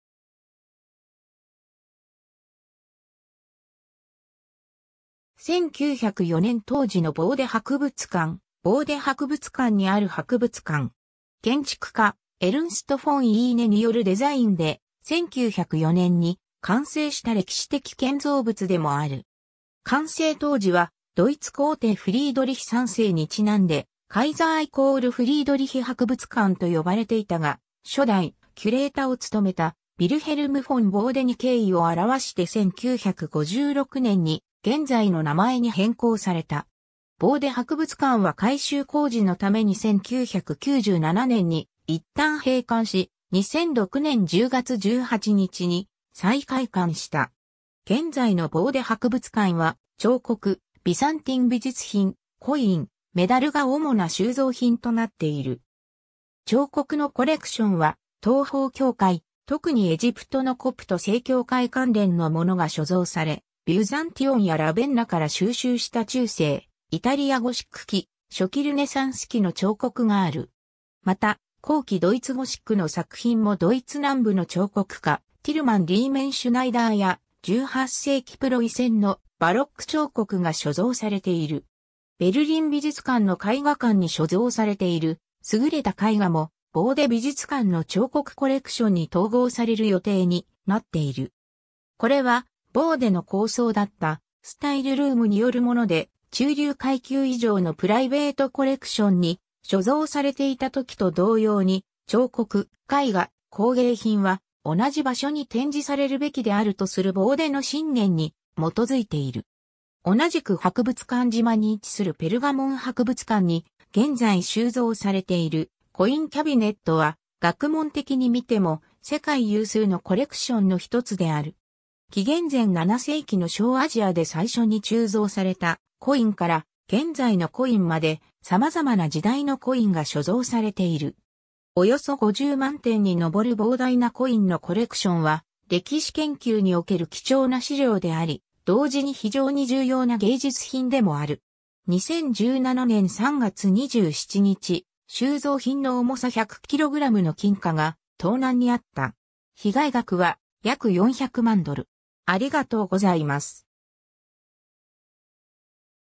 ボーデ博物館
ミッテ区_(ベルリン)
1904年当時のボーデ博物館 ボーデ博物館（ボーデはくぶつかん、独: Bode-Museum) はドイツ・ベルリンのムゼウムスインゼル（博物館島）にある博物館。
建築家エルンスト・ フォン・イーネによるデザインで、1904年に完成した歴史的建造物でもある。
完成当時はドイツ皇帝フリードリヒ3世にちなんでカイザー＝フリードリヒ博物館と呼ばれていたが、初代キュレータを務めたヴィルヘルム・フォン・ボーデに敬意を表して1956年に現在の名前に変更された。
 ボーデ博物館は改修工事のために1997年にいったん閉館し、2006年10月18日に再開館した。
現在のボーデ博物館は、彫刻、ビザンティン美術品、コイン、メダルが主な収蔵品となっている。
 彫刻のコレクションは東方教会、とくにエジプトのコプト正教会関連のものが所蔵され、ビュザンティオンやラヴェンナから収集した中世イタリアゴシック期、初期ルネサンス期の彫刻がある。
また、後期ドイツゴシックの作品もドイツ南部の彫刻家ティルマン・リーメンシュナイダーや、18世紀プロイセンのバロック彫刻が所蔵されている。
ベルリン美術館の絵画館に所蔵されている優れた絵画も、ボーデ美術館の彫刻コレクションに統合される予定になっている。
これはボーデの構想だった「スタイル・ルーム」によるもので、中流階級以上のプライベートコレクションに所蔵されていたときと同様に、彫刻、絵画、工芸品は同じ場所に展示されるべきであるとするボーデの信念に基づいている。
 同じく博物館島に位置するペルガモン博物館に現在収蔵されている「コイン・キャビネット (Münzkabinett)」は学問的に見ても世界有数のコレクションの一つである。
紀元前7世紀の小アジアで最初に鋳造されたコインから現在のコインまで、さまざまな時代のコインが所蔵されている。
およそ50万点にのぼる膨大なコインのコレクションは、歴史研究における貴重な資料であり、同時に非常に重要な芸術品でもある。
 2017年3月27日、収蔵品の重さ100kgの金貨が盗難に遭った。
被害額は約400万ドル。